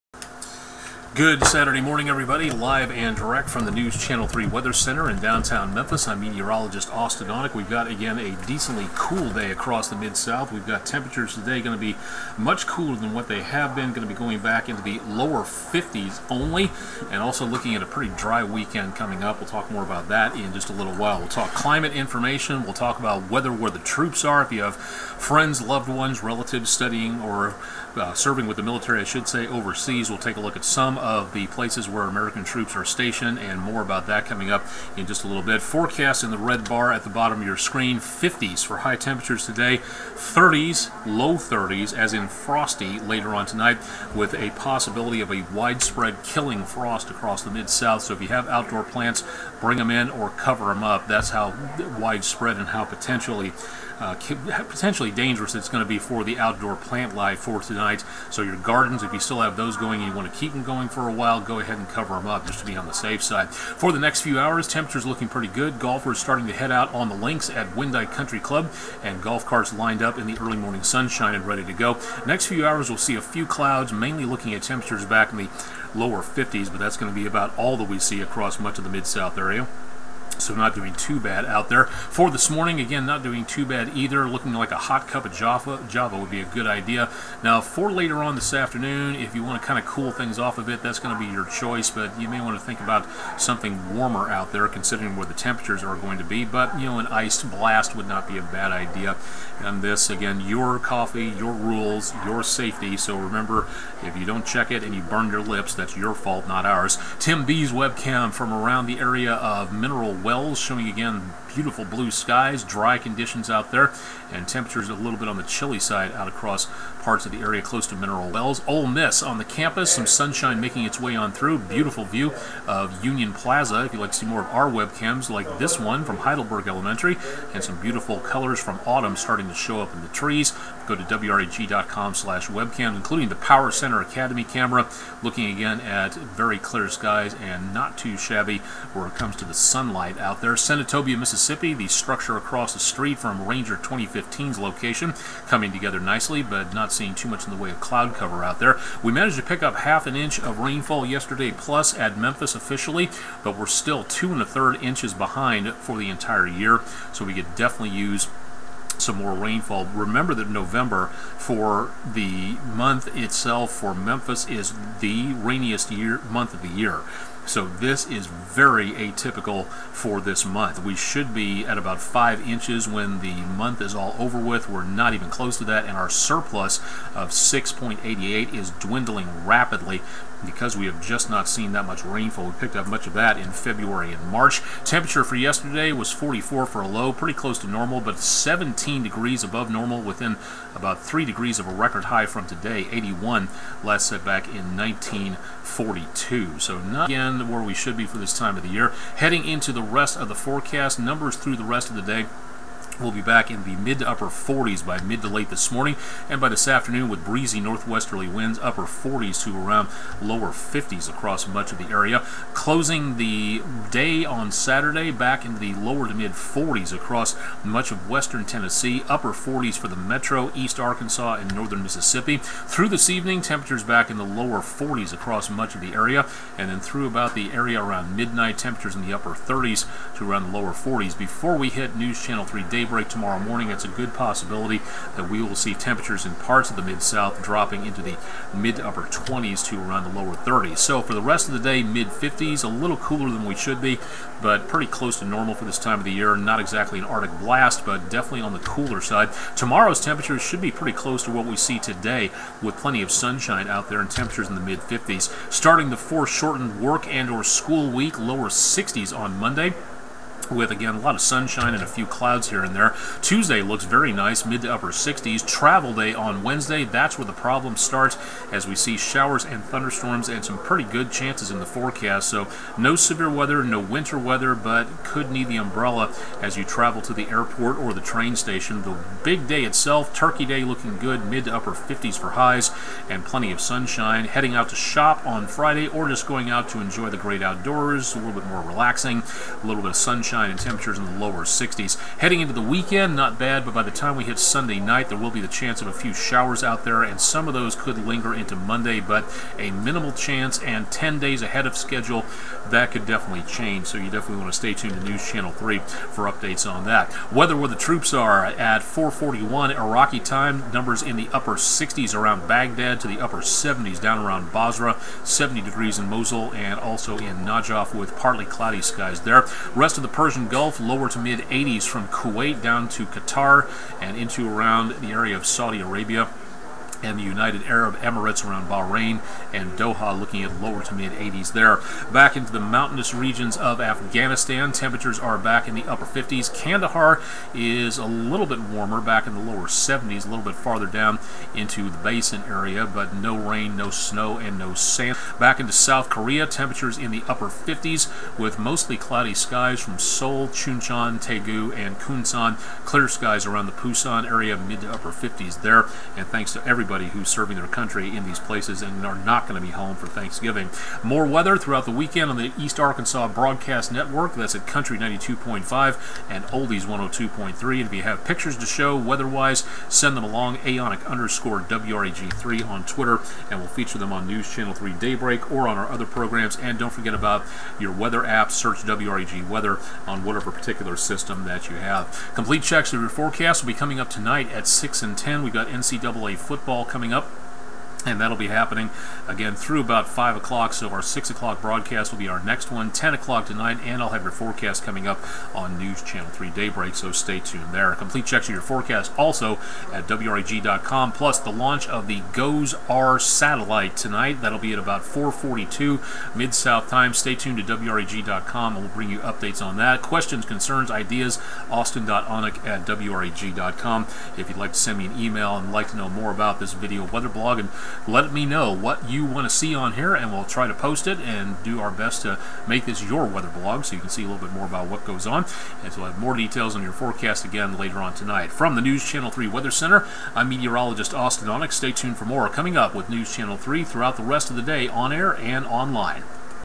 WREG-TV Weather Overtime for Saturday AM 11.19.16
WREG Meteorologist Austen Onek takes an in-depth look at the forecast for the Mid-South pre-Thanksgiving week.